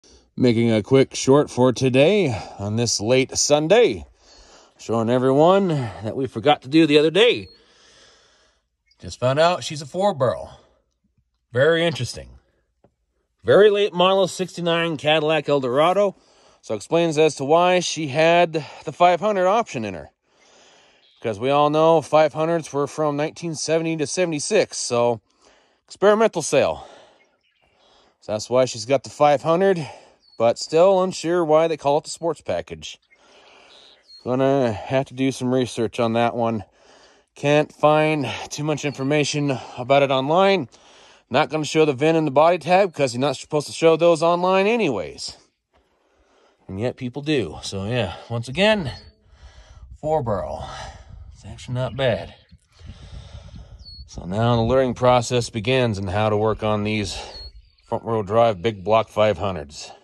The Eldorado Has A 4 Barrel!
Will be a more extensive video this coming Wednesday on the 1969 Cadillac Eldorado!

I couldn't wait around to show everyone due to us forgetting to check if this had a 2 barrel or 4 barrel and with great excitement upon my discovery, it's a factory 4 barrel!  A late model 1969 Cadillac Eldorado with a 500 4 barrel engine still intact!  Unsure if it's the original carb just yet but it's a 4 barrel!

The car will remain 100% original!